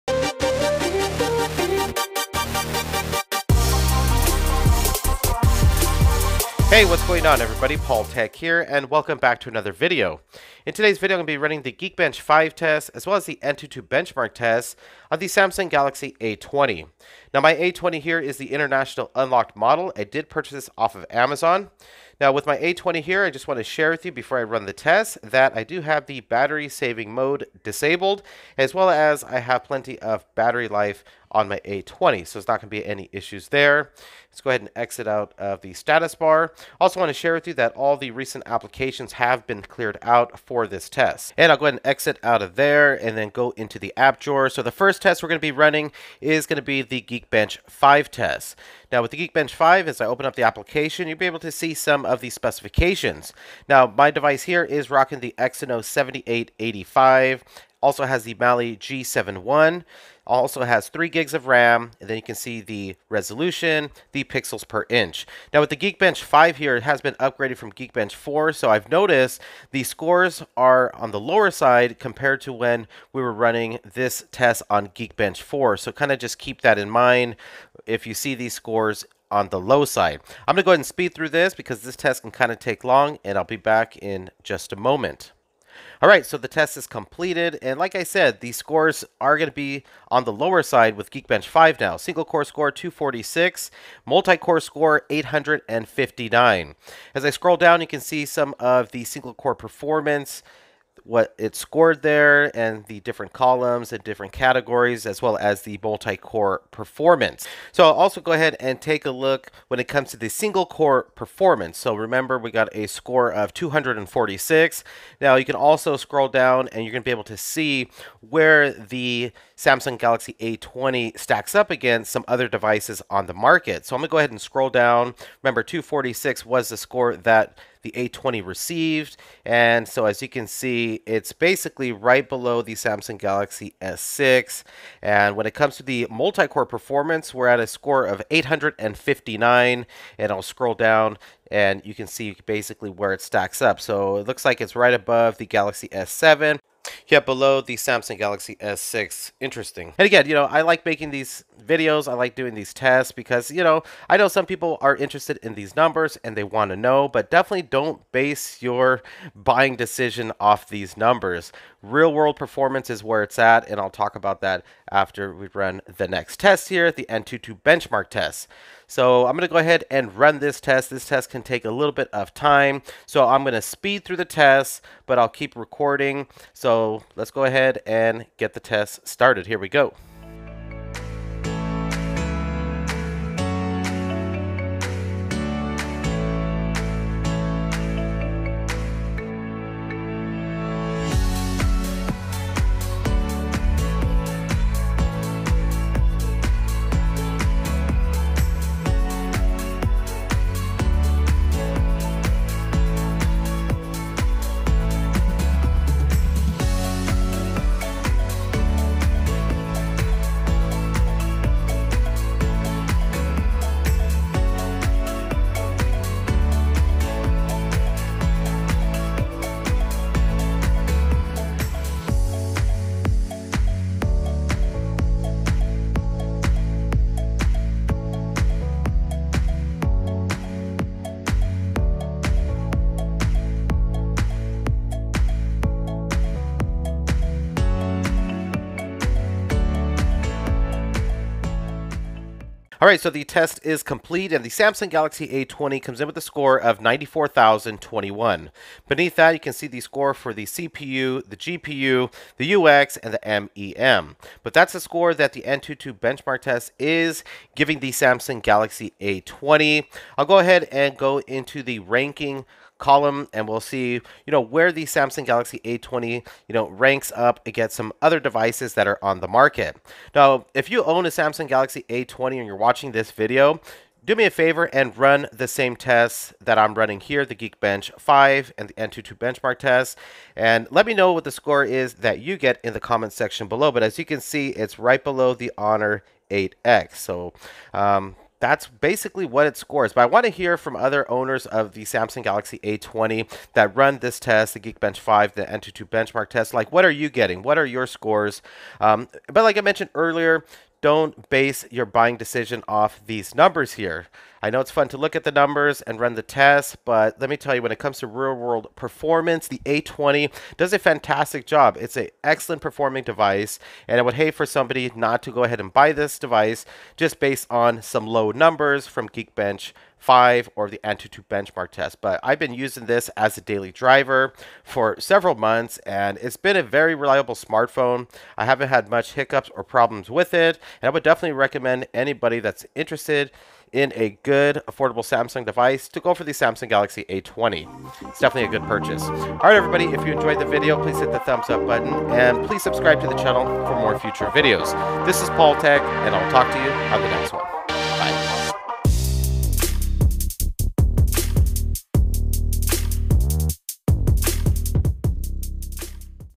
Samsung Galaxy A20 - Geekbench 5 and Antutu Benchmark Test
Welcome back to another video, todays video is a test. On the Samsung Galaxy A20 running Geekbench 5, and the Antutu Benchmark test.